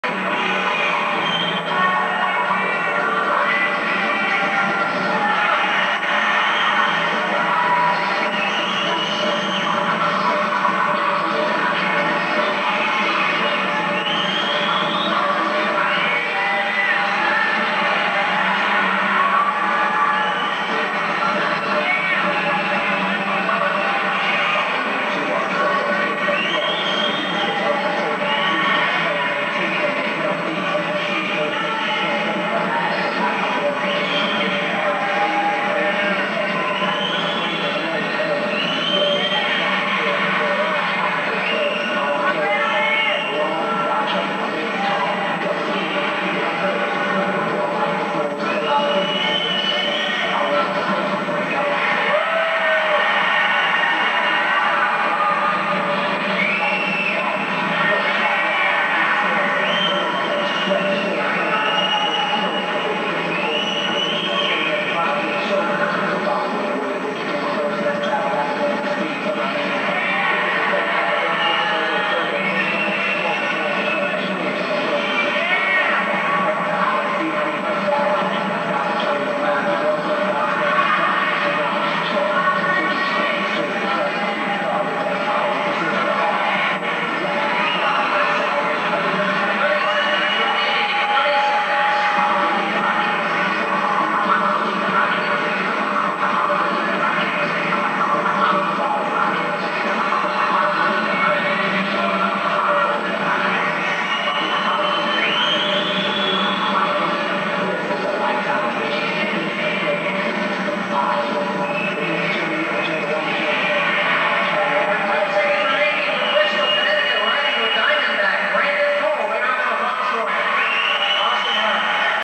Brandon Cole - Prelim - 1988 AFA Masters, Round 5, Wayne, NJ
Brandon Cole's 4th place 15 expert Prelim flatland bmx run, AFA Master Round 5, August 27, 1988. William Patterson College. Wayne, NJ.